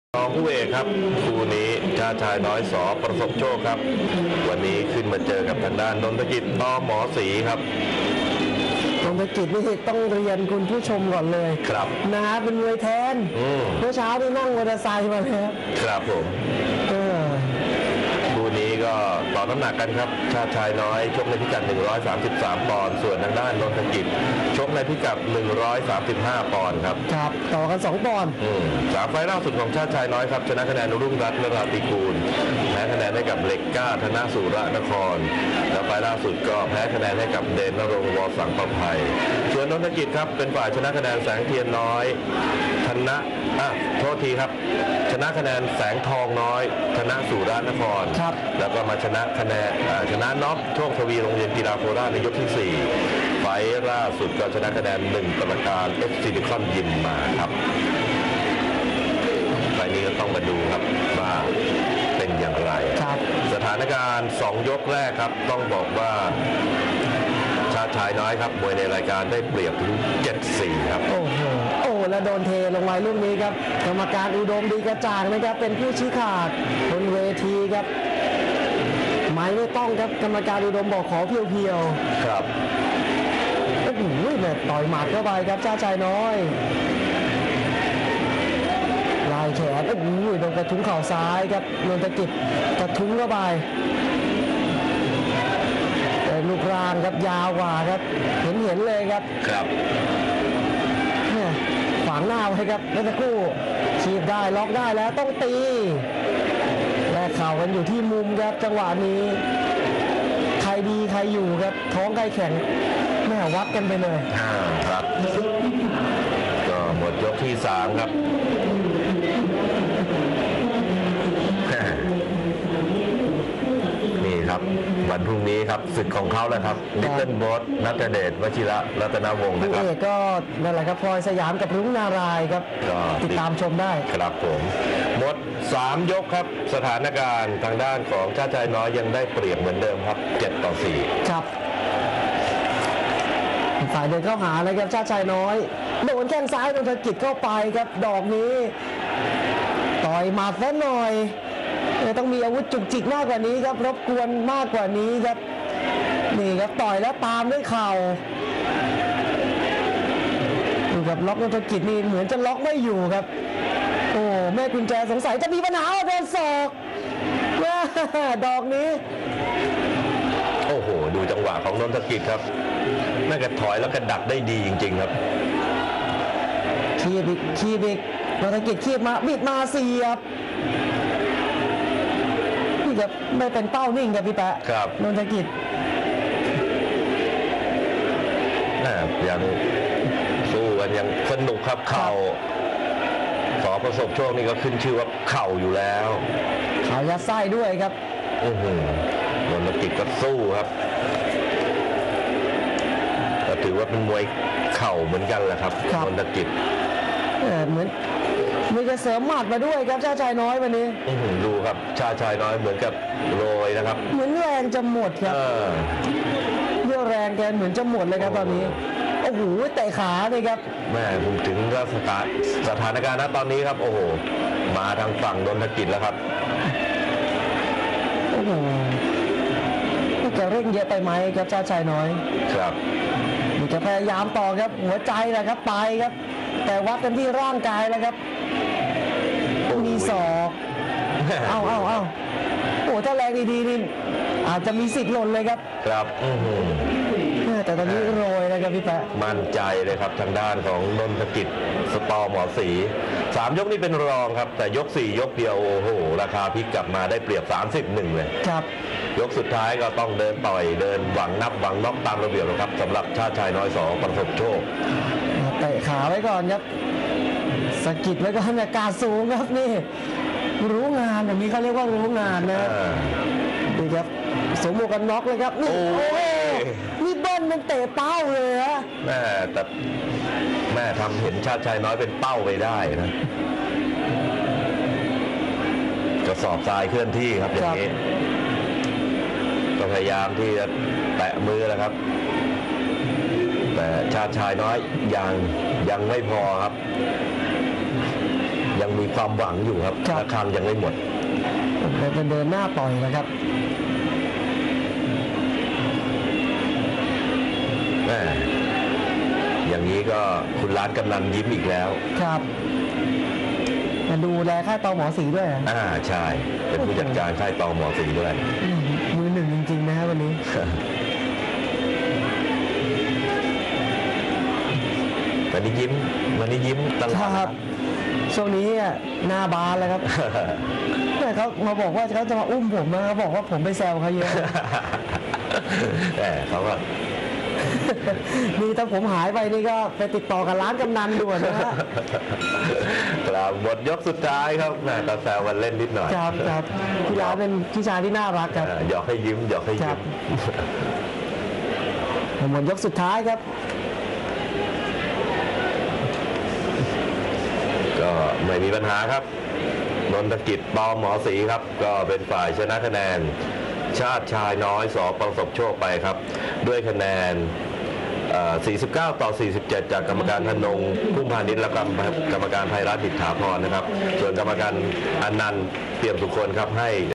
Chartchainoi Sor Prasopchok vs Nontakit Tor Morsri 11th June 2013
Chartchainoi Sor Prasopchok (red) vs Nontakit Tor Morsri (blue) at 133lbs & 135lbs. From Lumpinee Stadium, Bangkok on the 11th June 2013.

Chartchaynoi Sor.Prasobchok, real original muay thai boxing fights match highlights thailand , lumpini, lumphini, lumphinee